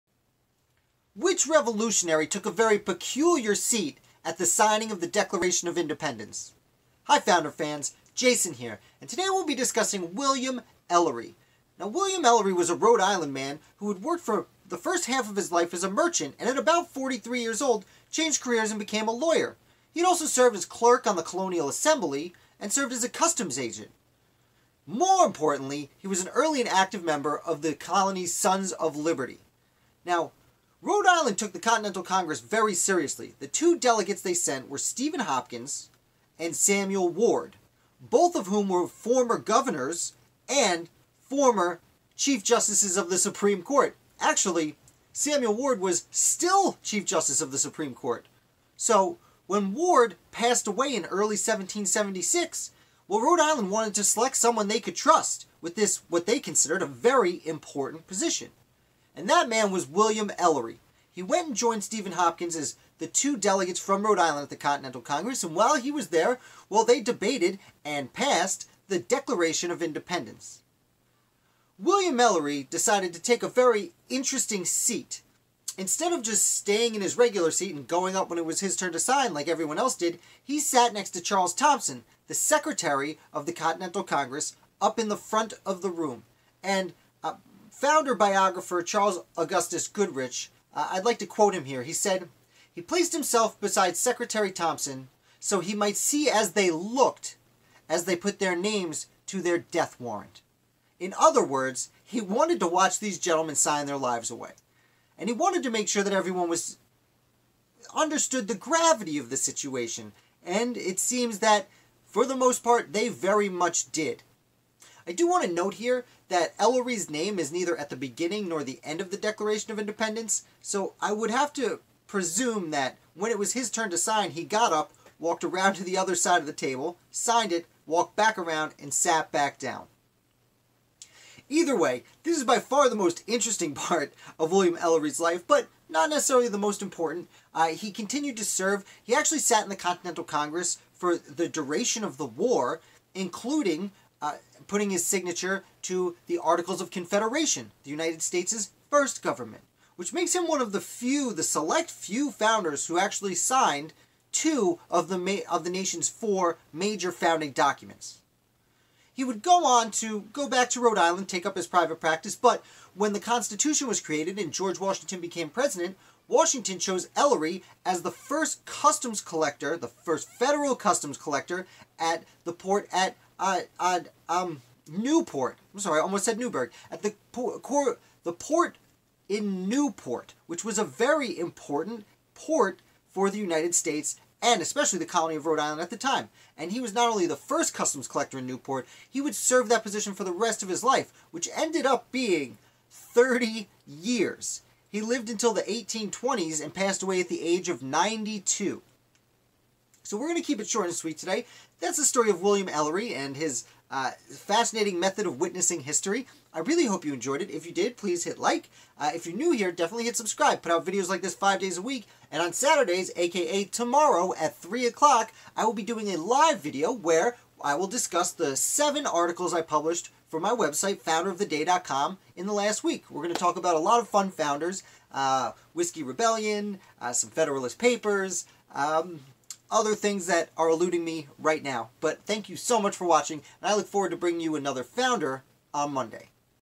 William Ellery Takes a Seat for the Signing
William Ellery was a signer of the Declaration of Independence who chose a very interesting spot to sit for that famous event.